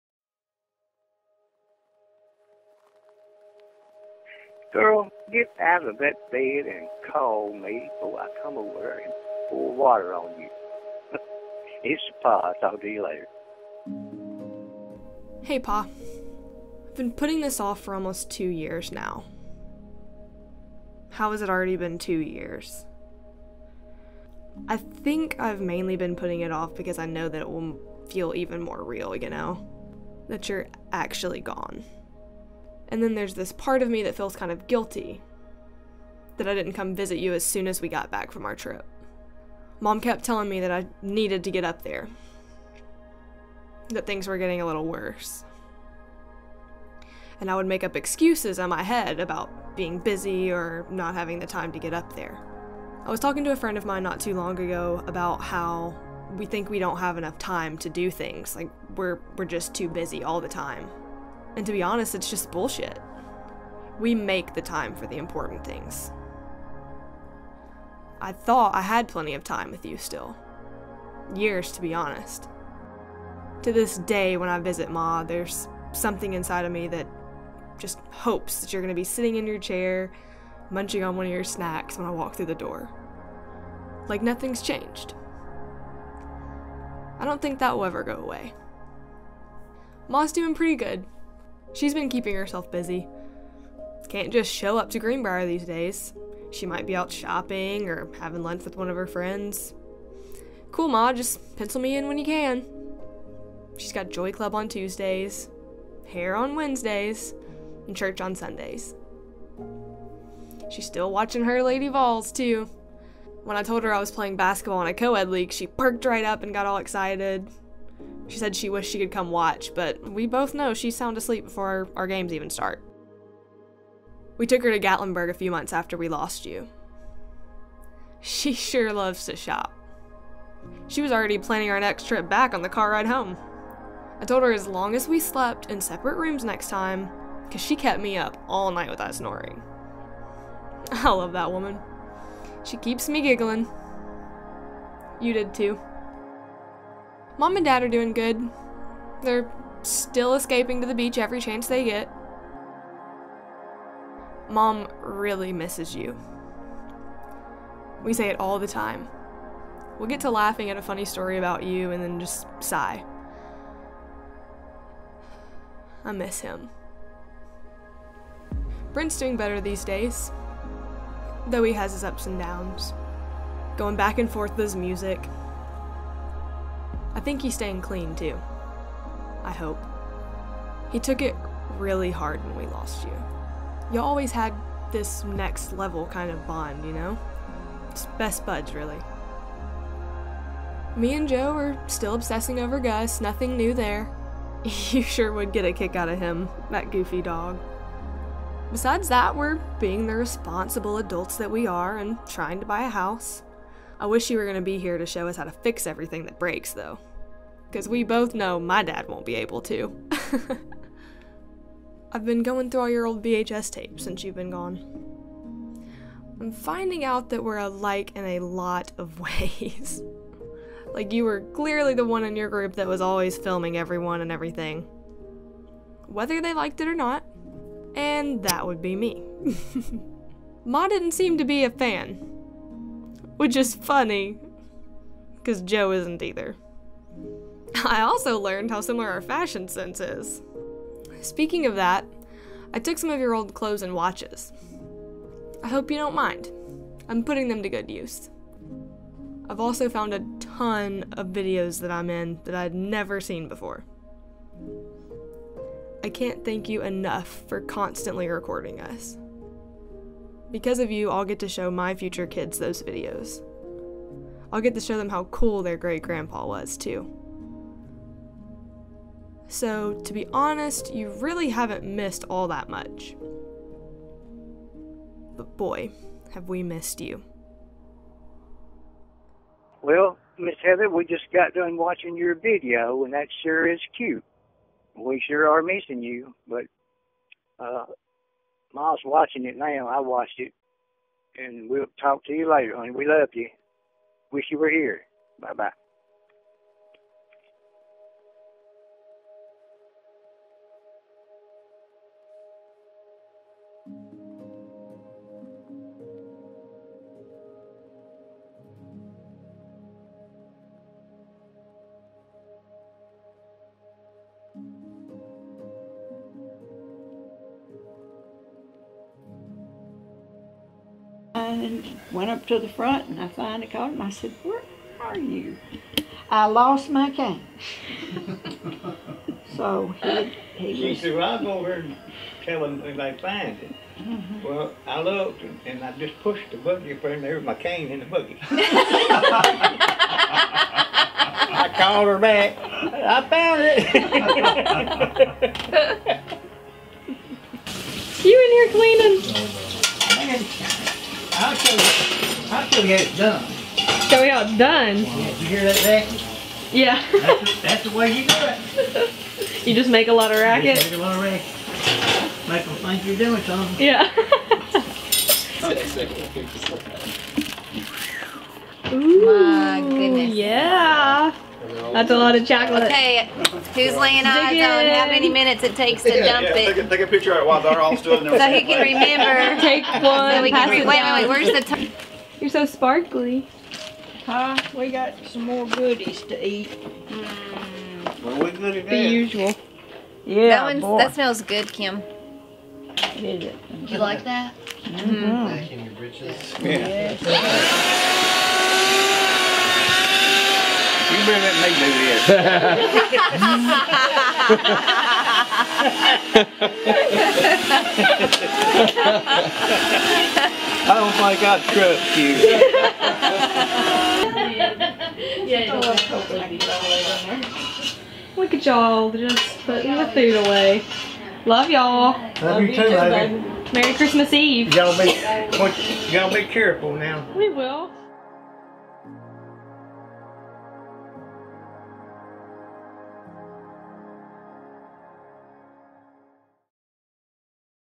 For Pa
Today, my Pa would have been 79 years old.

It's been almost 2 years since we lost him. Still doesn't feel real.